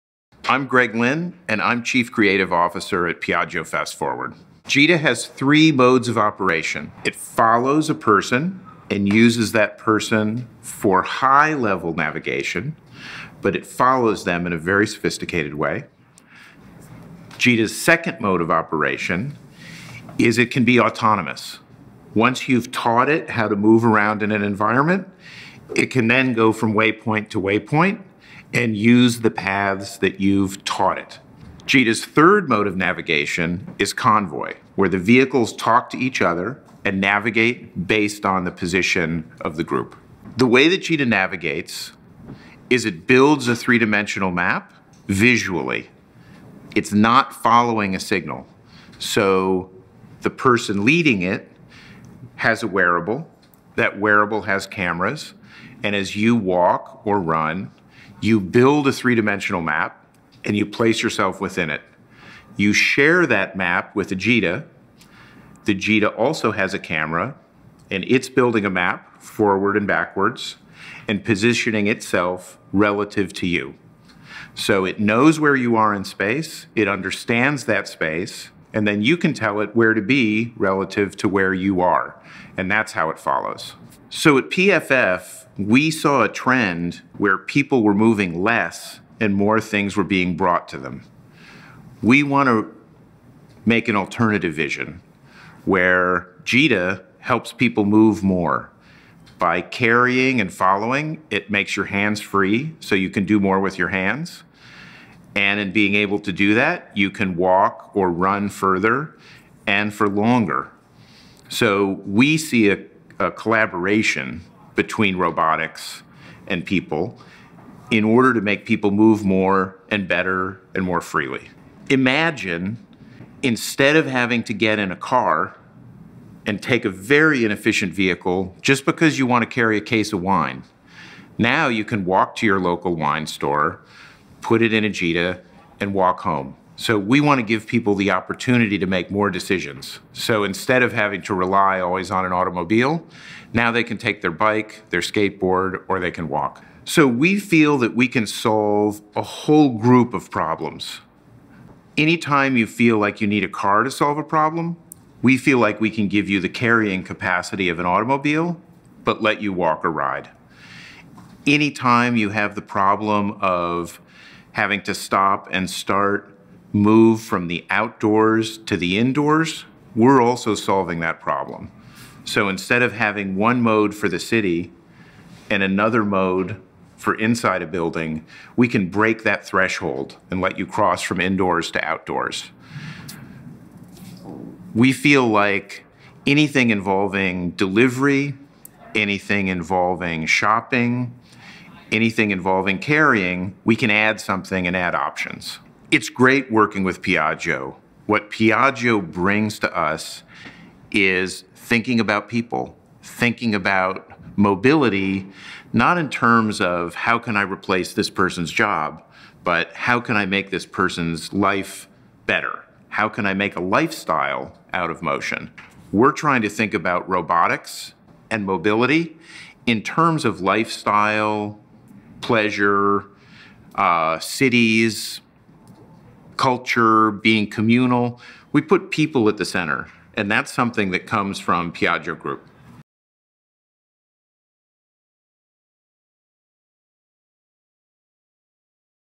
PFF - Greg Lynn
Greg Lynn (Chief Creative Officer of PFF) talks about GITA's modes of operations and Piaggio Fast Forward vision.
*Gita now operates seamlessly without a wearable belt!